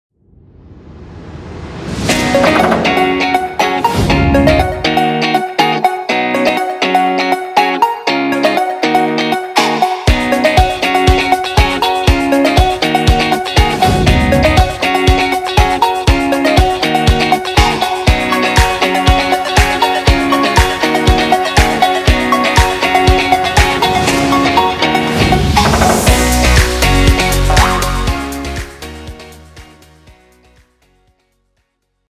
Ice Cream Bucket - Asobu
Blender 3D animation for Asobu Ice Cream Bucket.